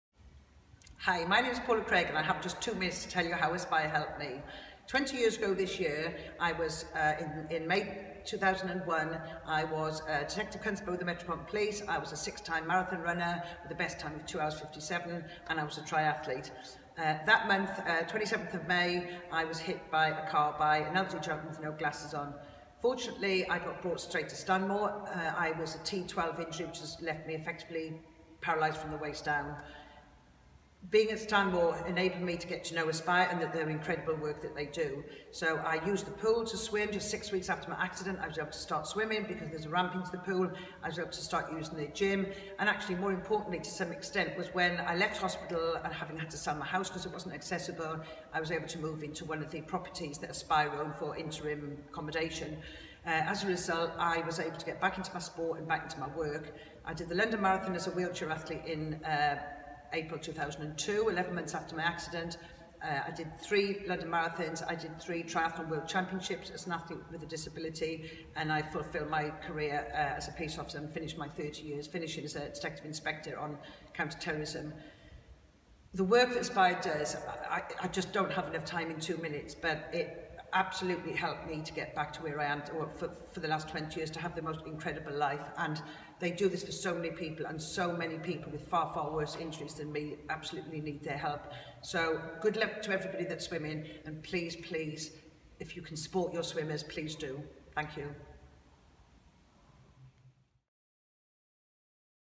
Paula Craig talks about life following her Spinal Cord Injury and how Aspire helped her
Aspire supporter Paula Craig was paralysed by a spinal cord injury 20 years ago and this year is taking on a relay channel swim.